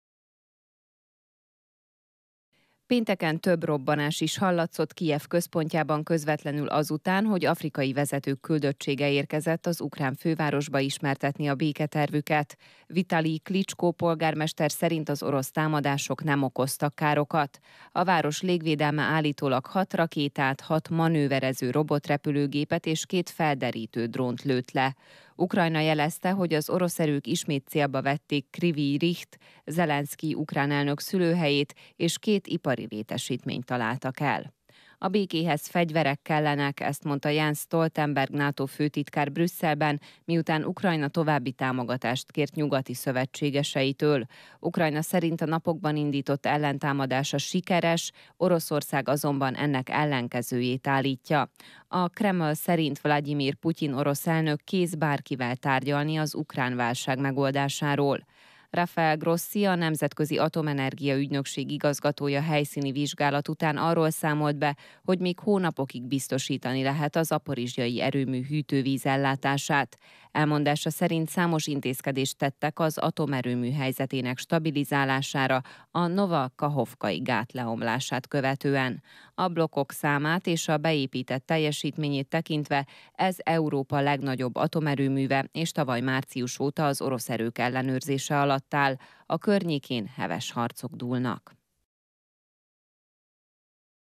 2023.06.16. - NATO-főtitkár: a békéhez fegyverek kellenek
Ukrajna azt állítja, hogy az ellentámadás során több mint 100 négyzetkilométernyi területen szerezte vissza az ellenőrzést Donyec és Zaporizzsja térségében.